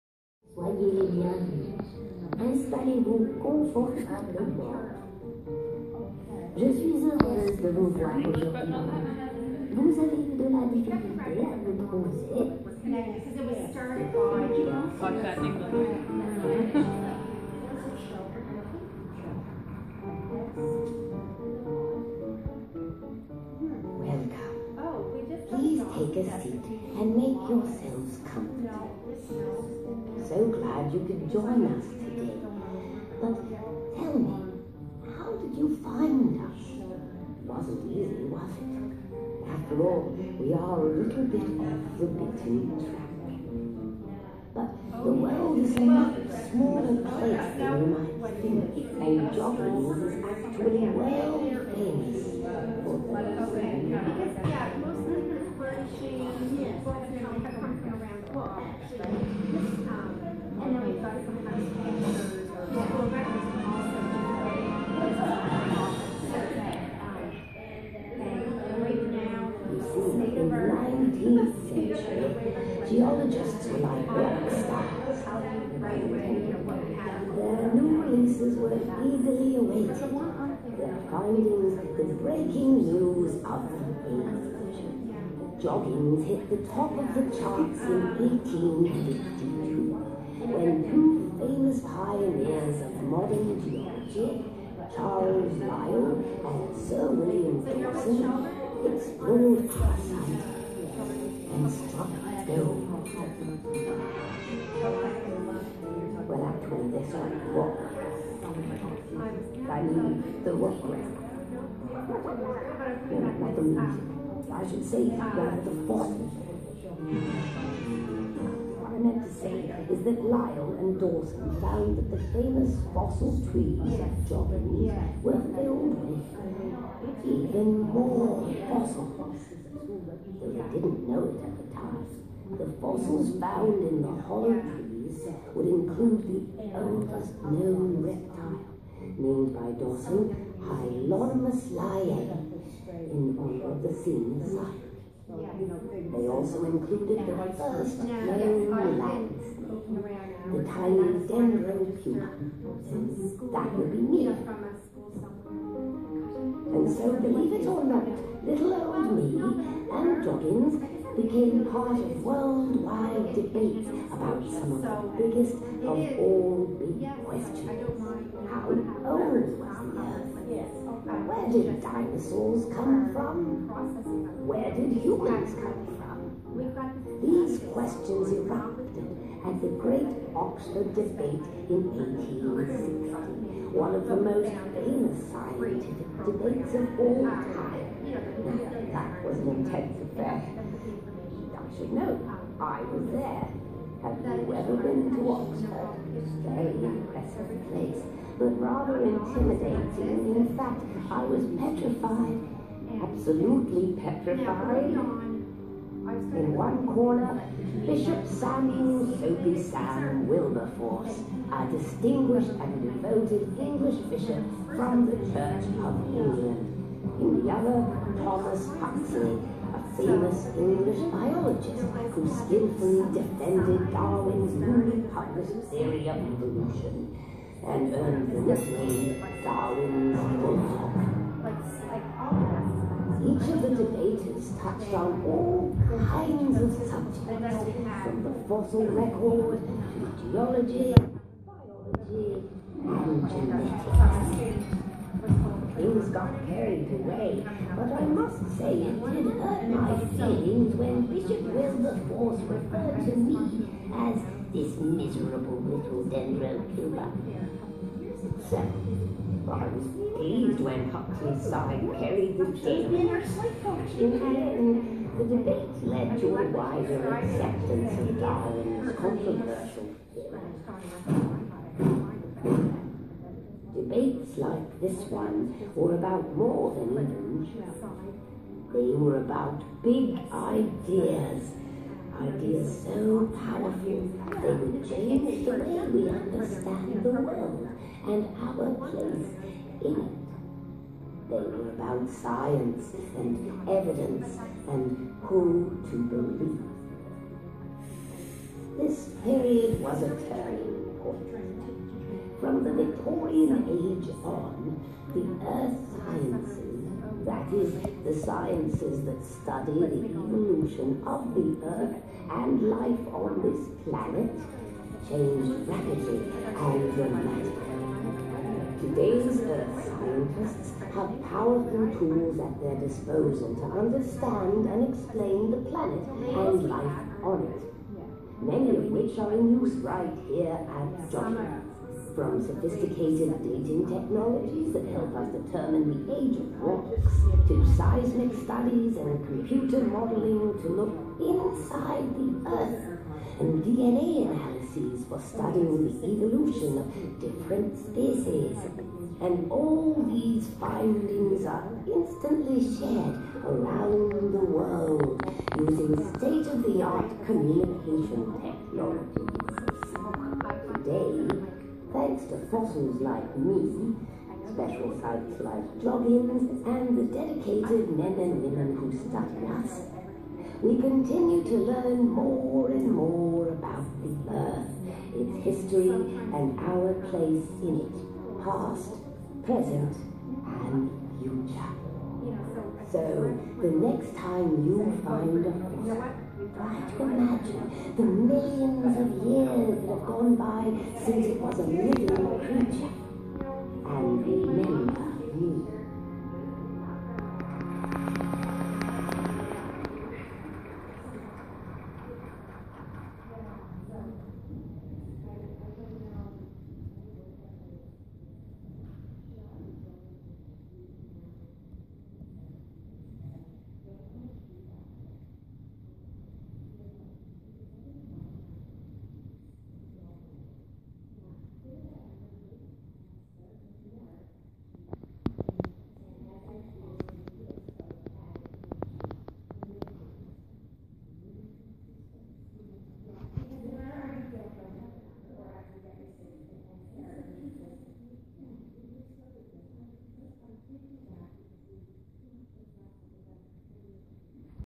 Joggins Fossil Cliffs video presentation
I apologise for the orientation! I was just planning to catch the French part for a short Instagram video.